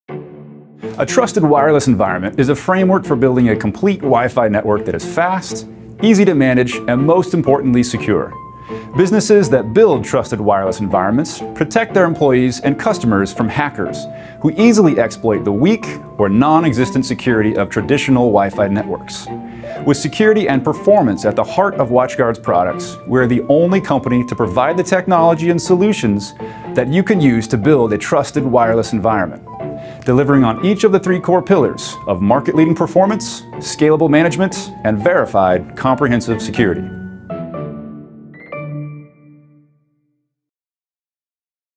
TWE Overview Video (CZ titulky)
Důvěryhodné bezdrátové prostředí TWE, nenechte Wi-Fi být vaší největší bezpečnostní mezerou.